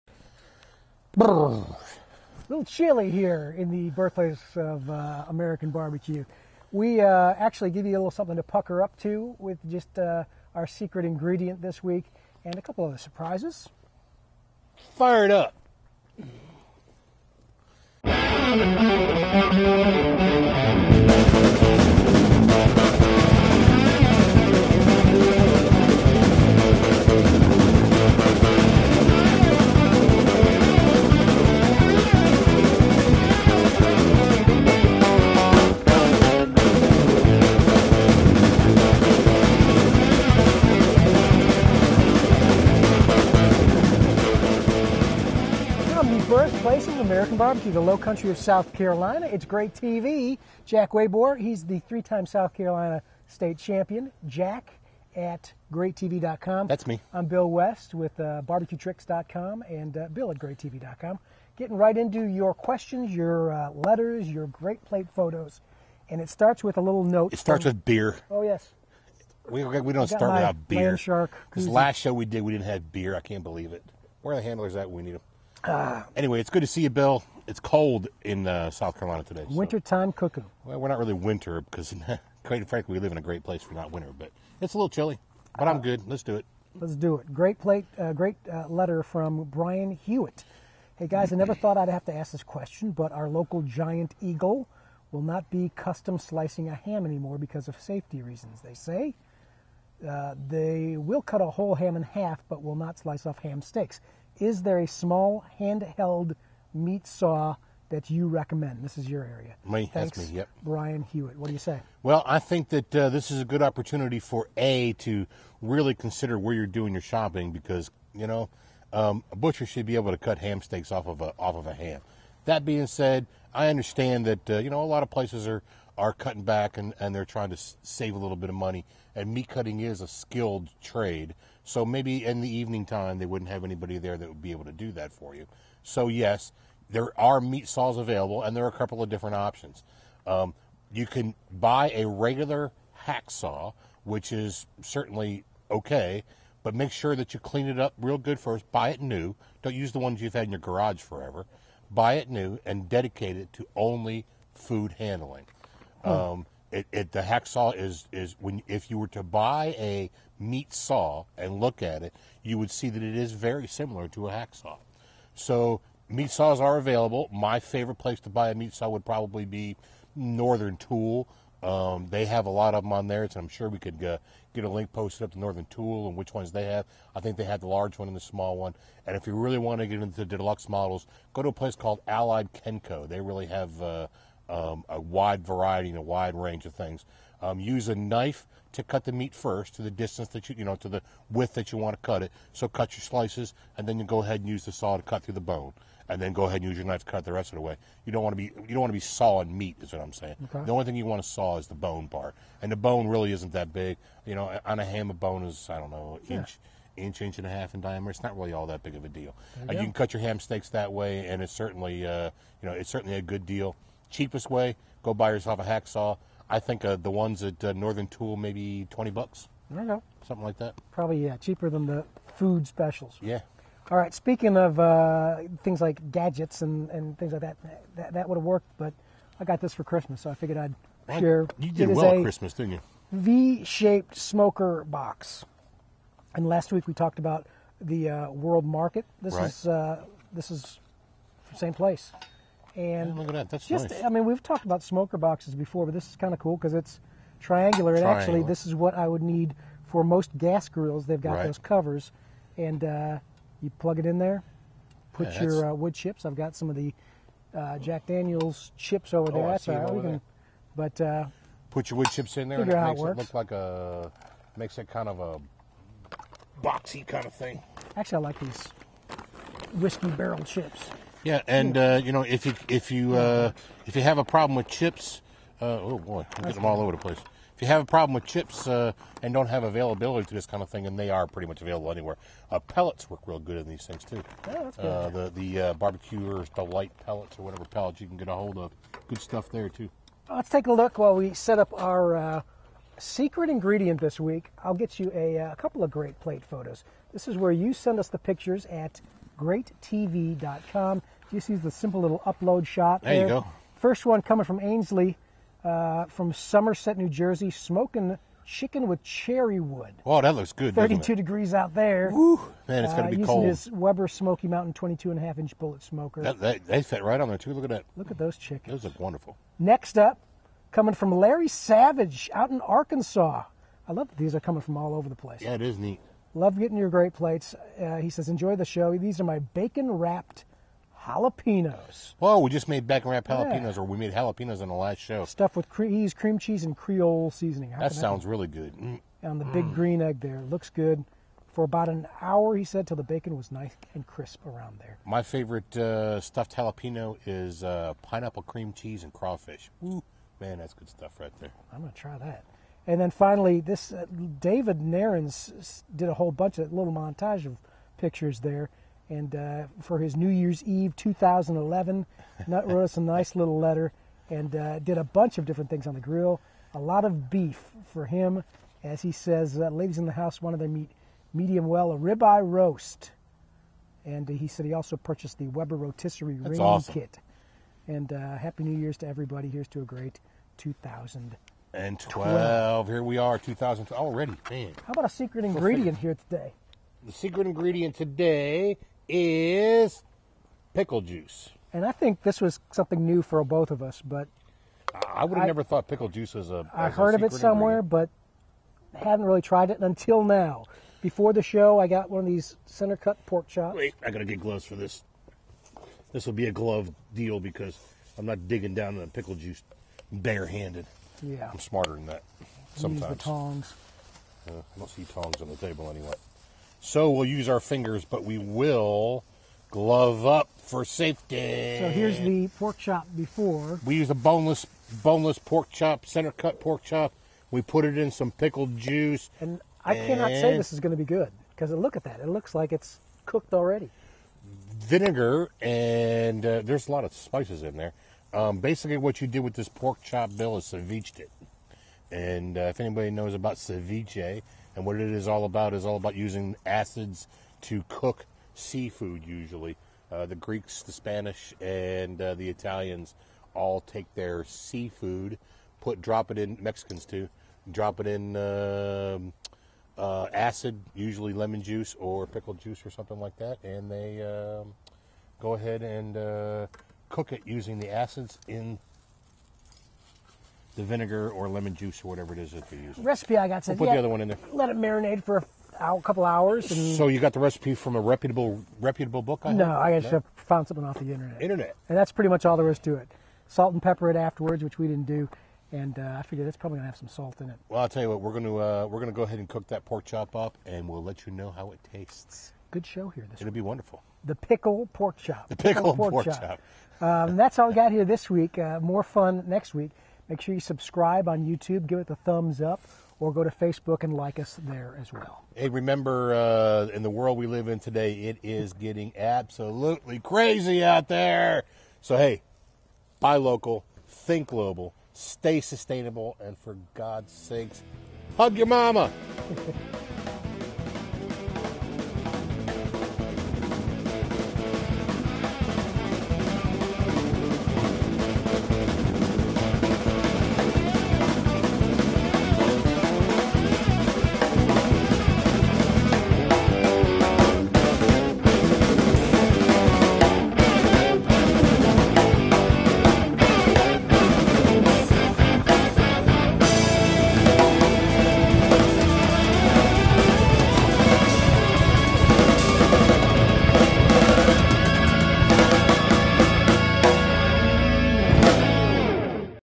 V-Shaped Smoker Box, Pickled Pork and Grate Plates
a triangular smoker box for gas grills and using pickle juice to marinate pork.  GrateTV is a BBQ Grill show that is about a beer in length.  Hug your Momma.